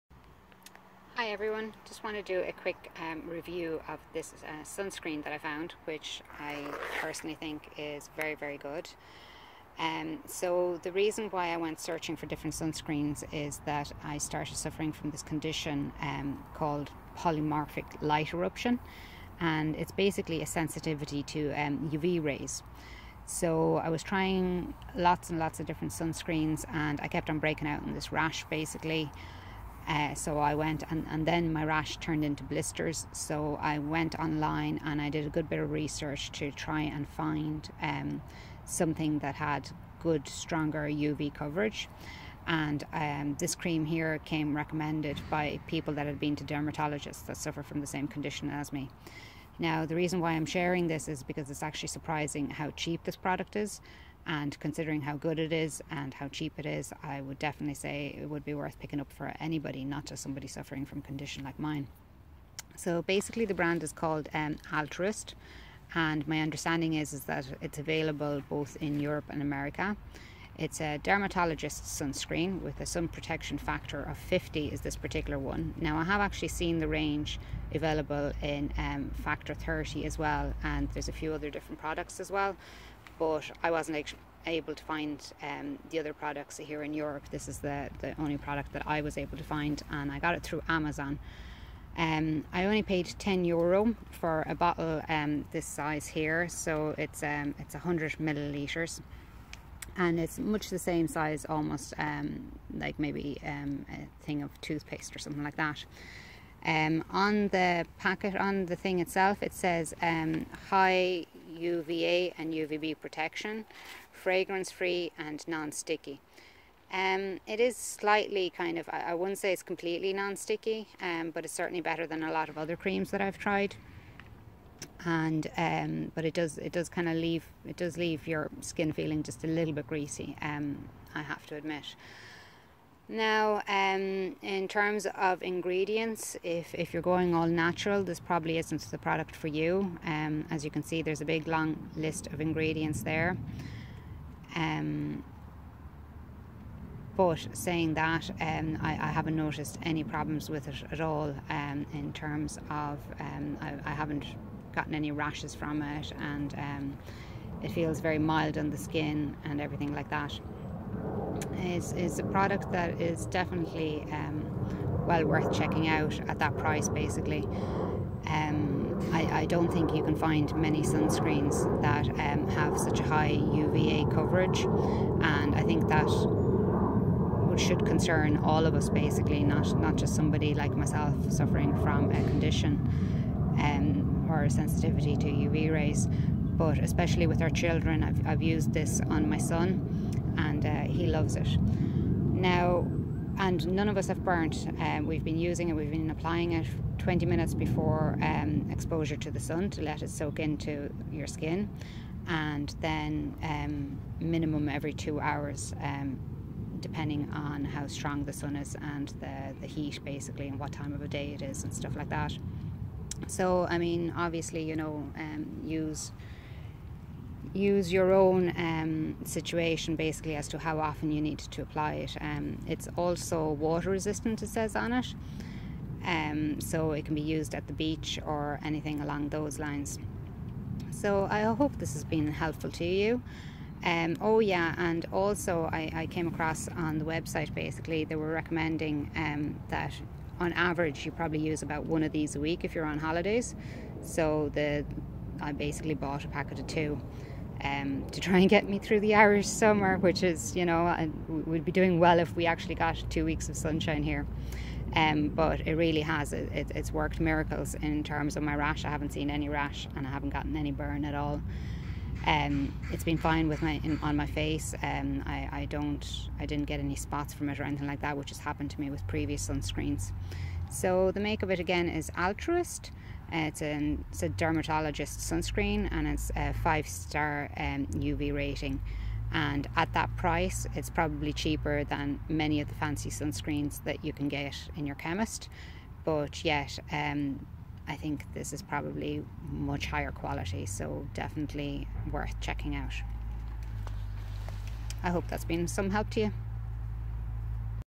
Altruist Dermatologist Sunscreen Review - PMLE, sun rash cream
Altruist is a specially developed sunscreen with a broad UVA and UVB spectrum coverage